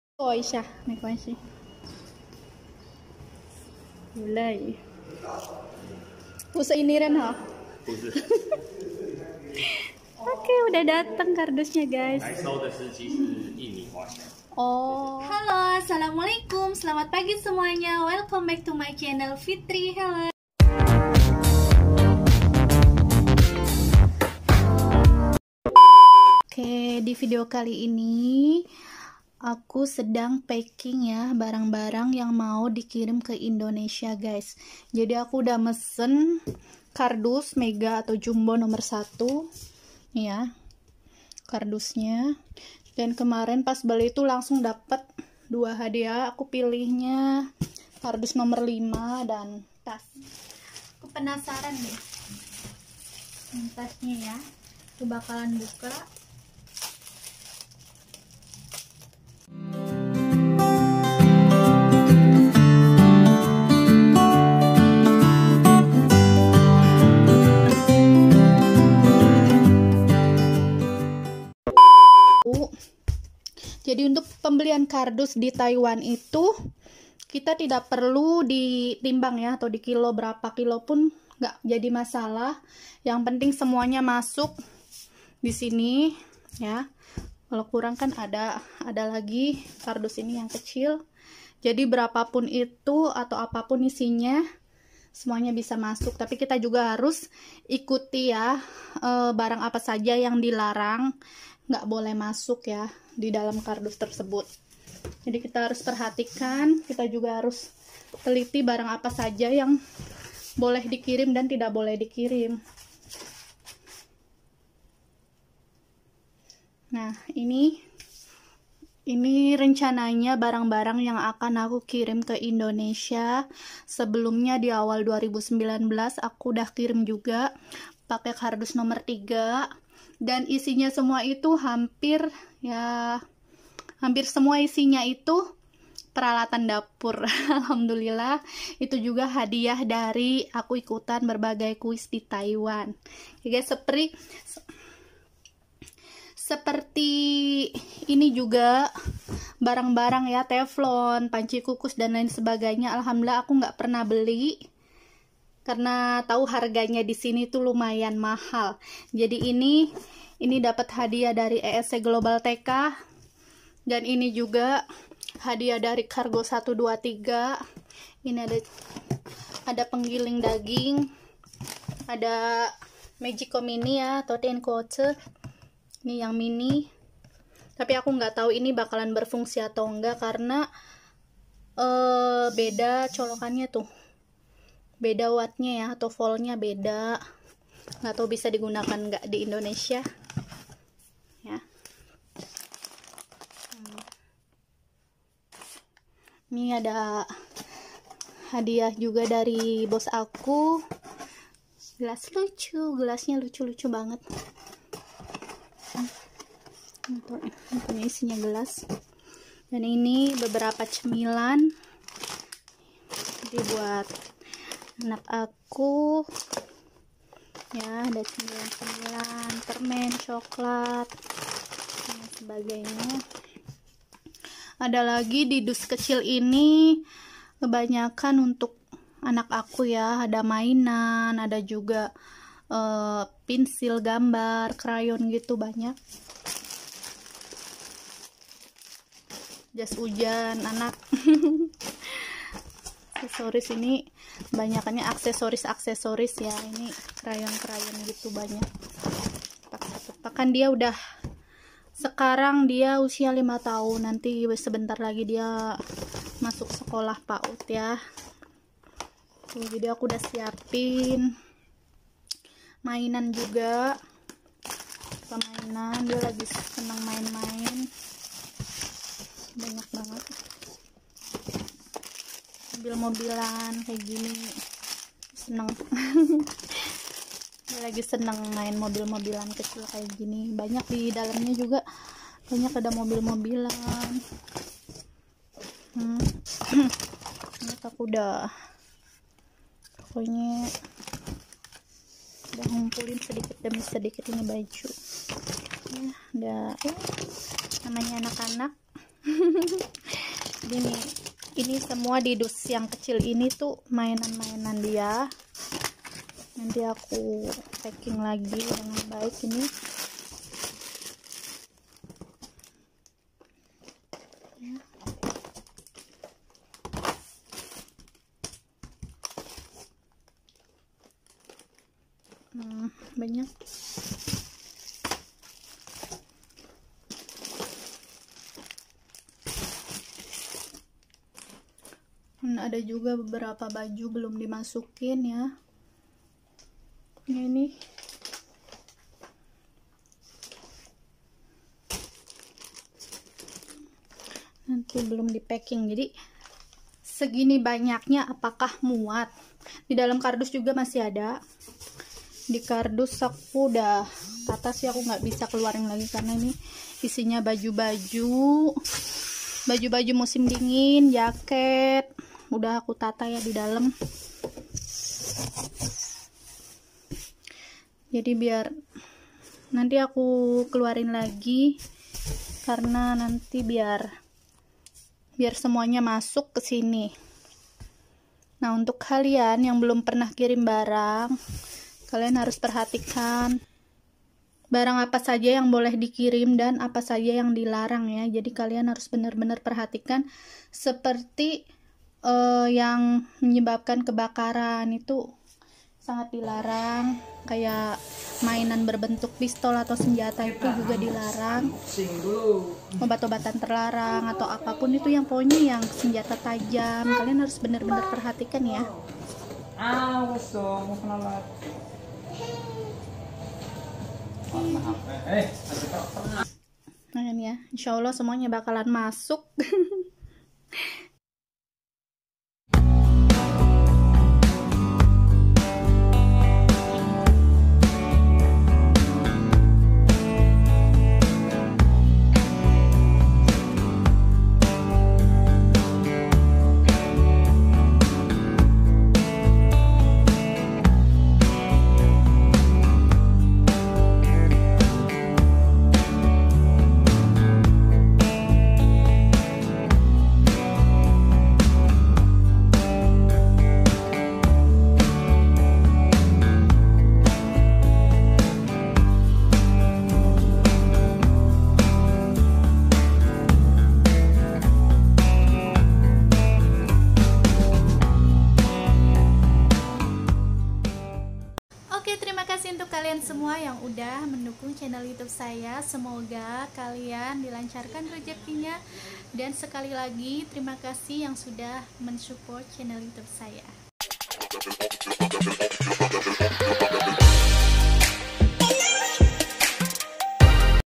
Cara Aman dan mudah Kirim Barang dari Taiwan ke Indonesia
Video ini adalah dokumentasi saat saya packing barang-barang Hadiah hasil ikutan kuis di Taiwan.
Agar barang tetap aman sampai ke Indonesia kita harus perhatikan beberapa cara packing yang benar dan aman.